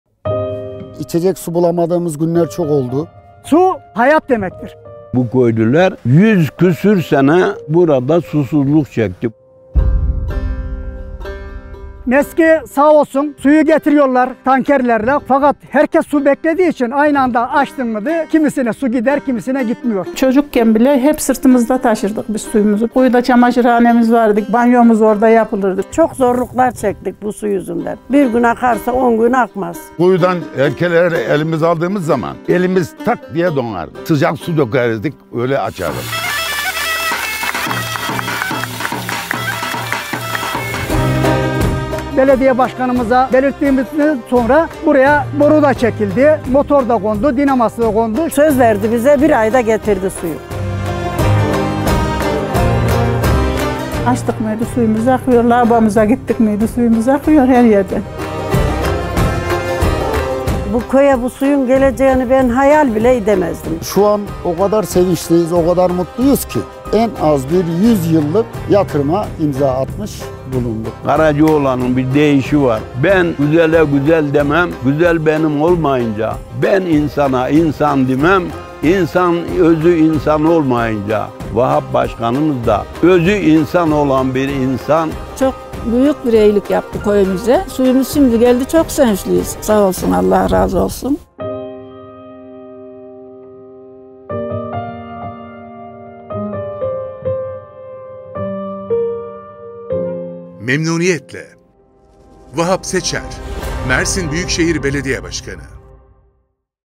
Ayaştürkmenli'ye asırlık bir sevinç yaşattık.Her şey Mersin için.
Ayaştürkmenli'ye asırlık bir sevinç yaşattık.
Her şey Mersin için.